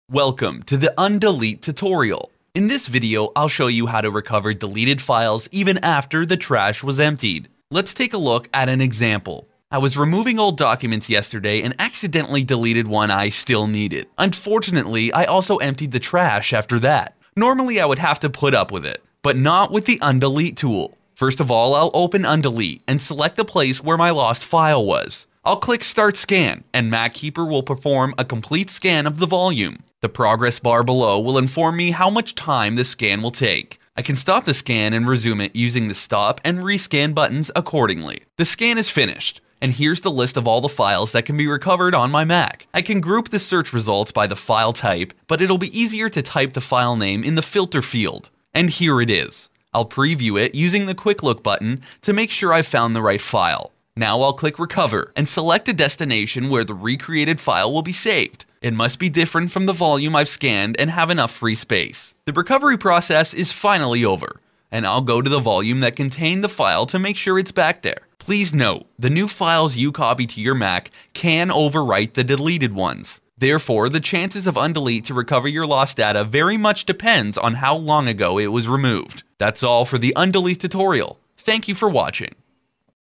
Undelete: MacKeeper Tutorials
This tutorial will show you how to recover deleted files even after the Trash was emptied.